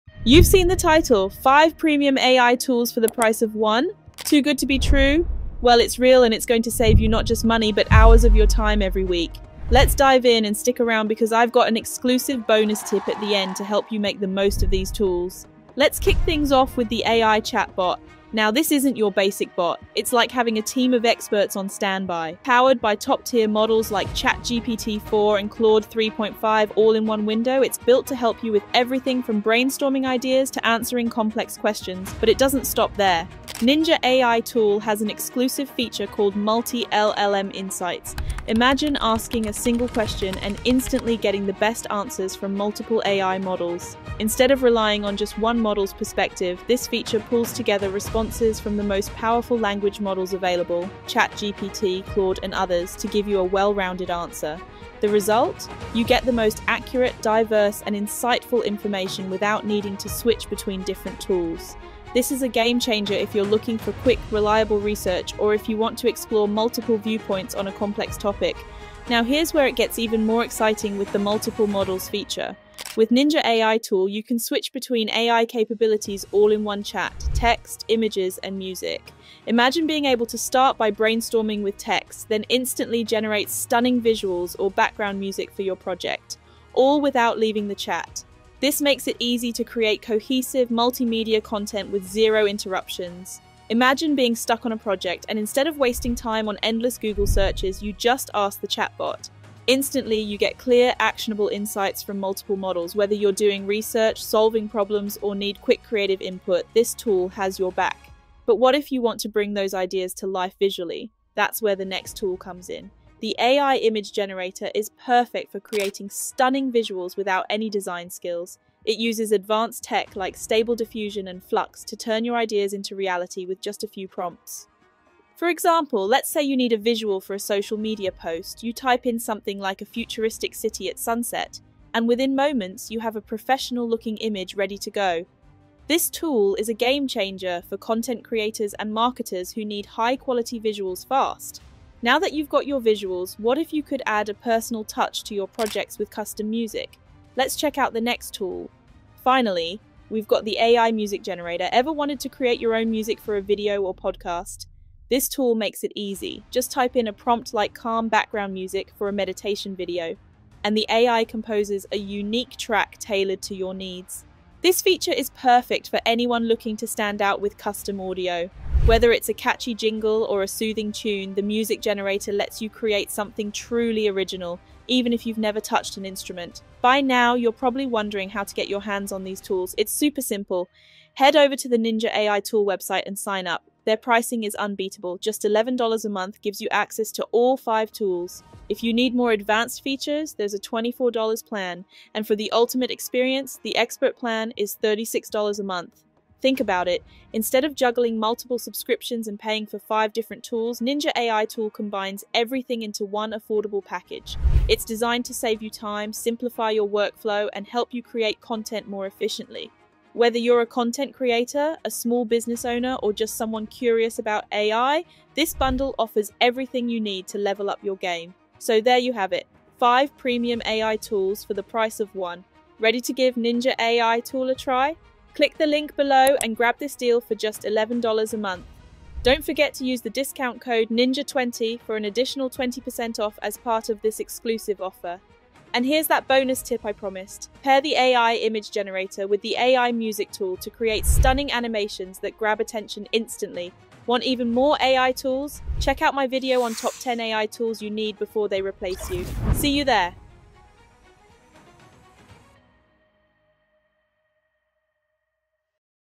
5 Premium AI Tools – All for the Price of ONE ?
Get access to 5 premium AI tools for the price of one! Boost your productivity and streamline your business with the best AI tools on the market.

✔️Time Stamp:
0:00 - 0:17 5 Premium AI Tools
0:18 - 0:38 AI chatbot features
0:39 - 1:00 Multi-LLM Insights explained
1:01 - 1:58 Multiple Models feature
1:59 - 2:54 AI image generator
2:55 - 3:09 AI music generator
3:10 - 3:49 NinjaAItool pricing
3:50 - 4:00 bonus tip